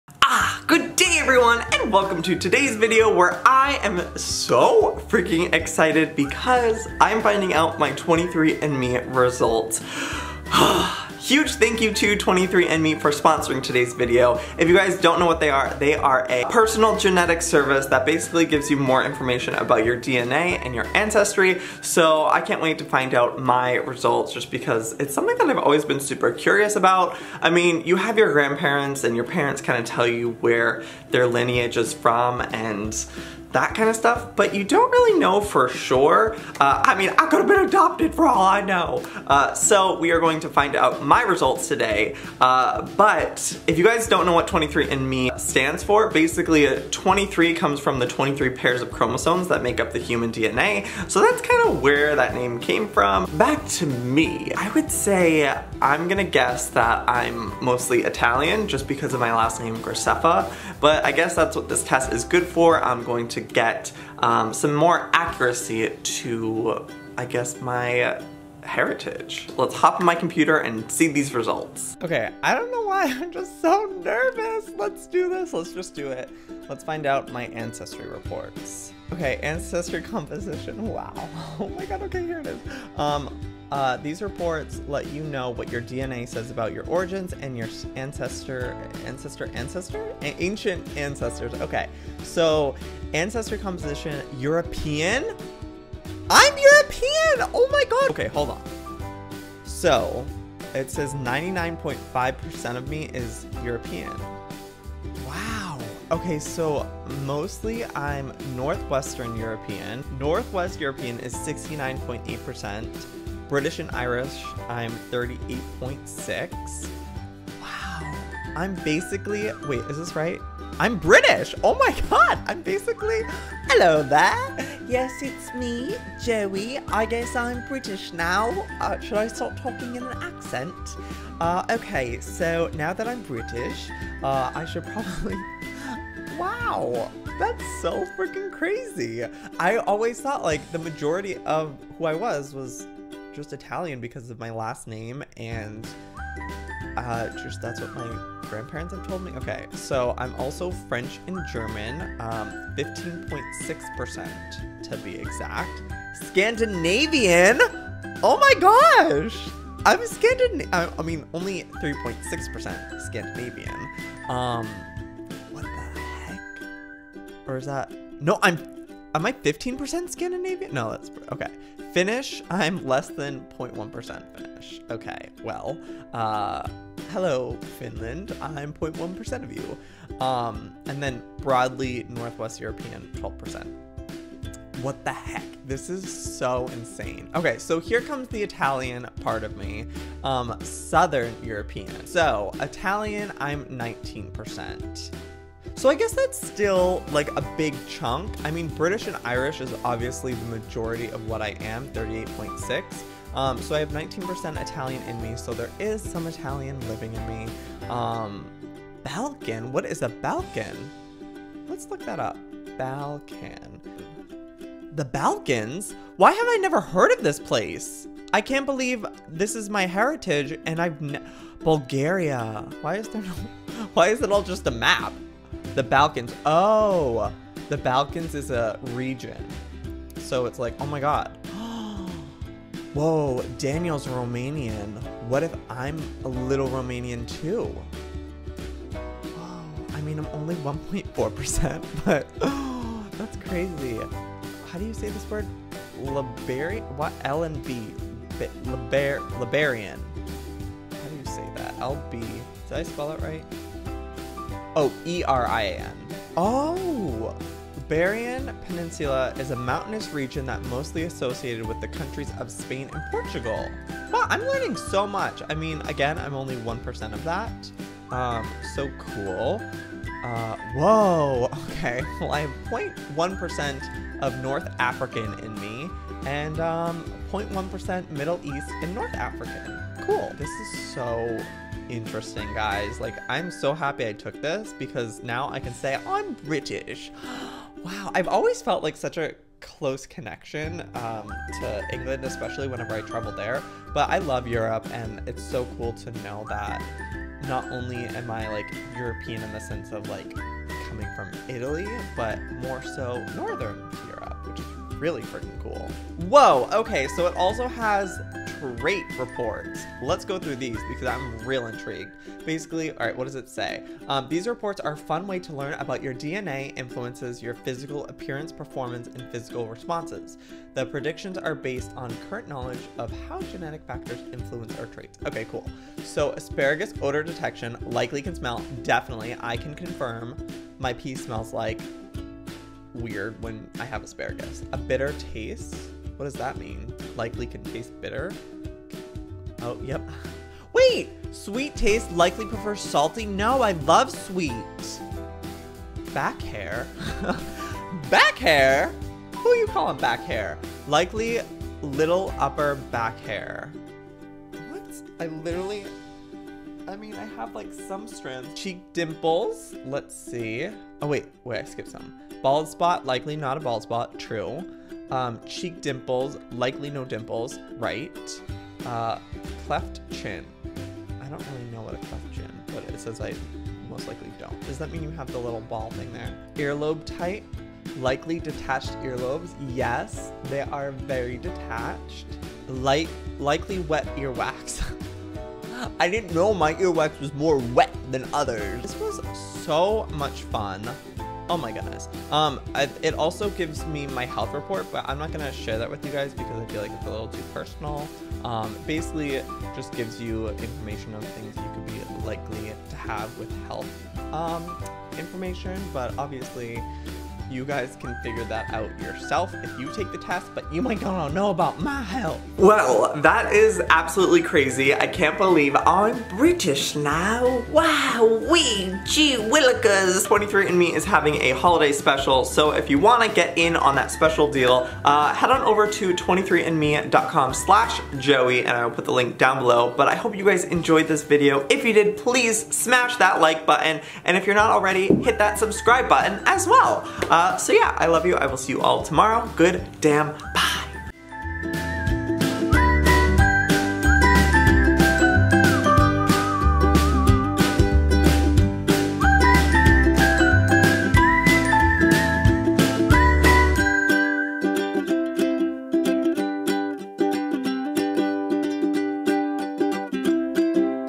The Truth About my Past...
Today i finally discover and reveal the truth about my past and share with you my reaction to my shocking 23 and me DNA results.